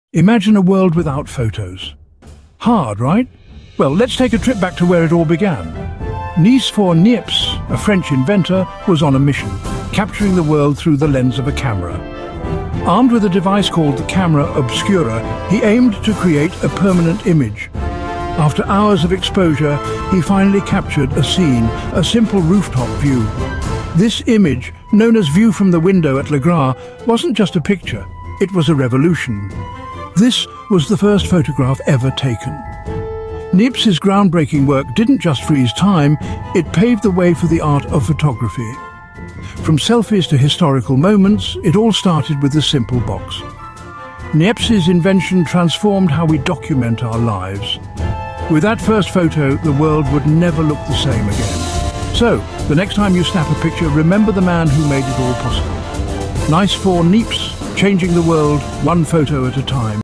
The First Photograph: Niépce's Revolutionary Invention| Historical Fact | First Camera| Unknown Fact
The Brief History of the Photography Camera
Ever wondered how they photographed ages ago? Watch this video and find out!

Cameras, among other things that we own and use frequently, play an important role in our daily lives. We use this small tool to capture great moments in our lives in the form of photographs, which means a lot to emotional beings like us.
However, the camera that we use today, whether as a small module on our smartphones, compact point and shoot cameras, or the mighty DSLRs, took a long time to develop.

The first camera:
The camera obscura was the first camera known to history. Conceptual descriptions of the cameraobscura can be found in Chinese texts dating back to 400 B.C. and in Aristotle's writings around 330 B.C. The Arab scholar Ibn Al-Haytham articulated the concept of a camera obscura around 1000 A.D. A camera obscura does not take pictures; instead, it focuses light through a lens (technically a small hole) and projects it onto a screen. pinholecamera are similar to camera obscuras. These devices paved the way for everything from still photography to movie cameras and projectors.